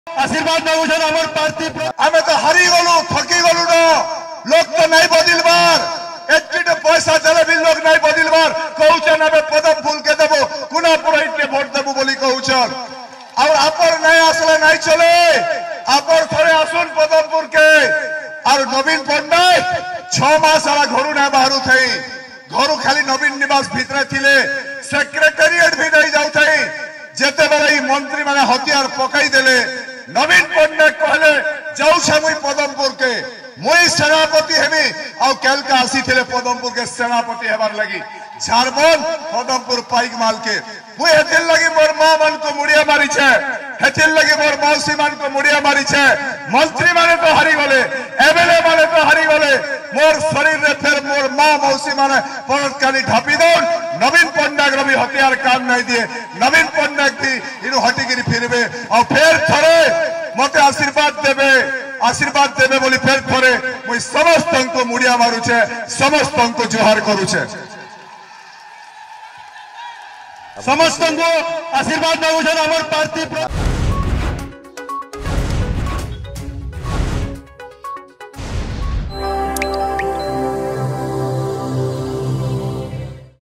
Padampur By Election | ଏଠି ରାଜ୍ୟ ସରକାରଙ୍କ କୌଣସି ହତିଆର କାମ କରିବନି
ଏଠି ରାଜ୍ୟ ସରକାରଙ୍କ କୌଣସି ହତିଆର କାମ କରିବନି : ପ୍ରଦୀପ ପୁରୋହିତ,ବିଜେପି ପ୍ରାର୍ଥୀ